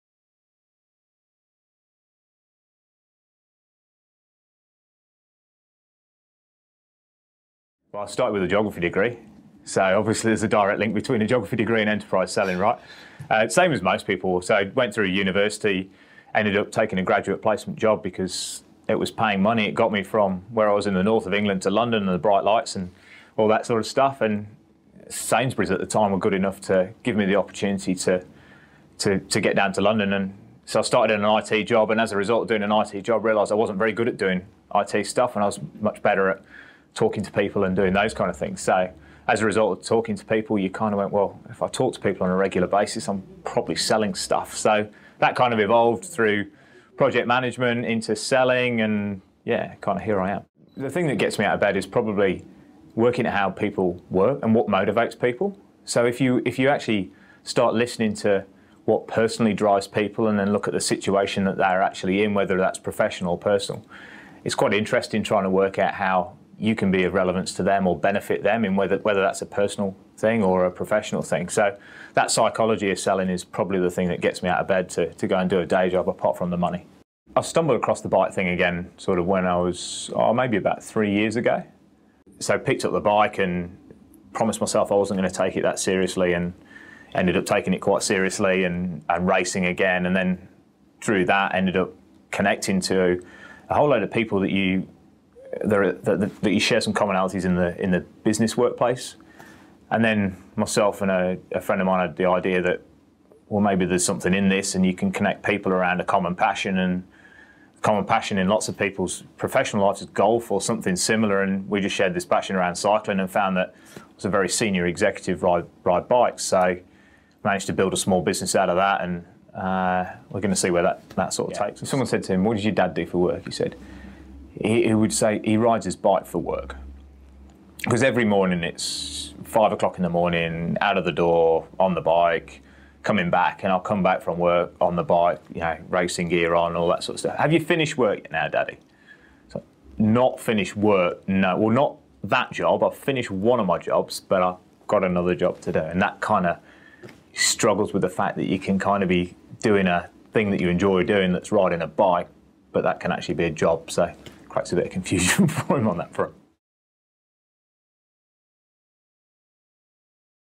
Client story: Gareth Williams, Strategic Account Executive, Salesforce.com
In this short film, Gareth Williams shares with us a little about his story, what he is passionate about and what makes him tick. By watching this video, it will also give you a feel for why Gareth has chosen Jeff Thurecht as his Financial Planner.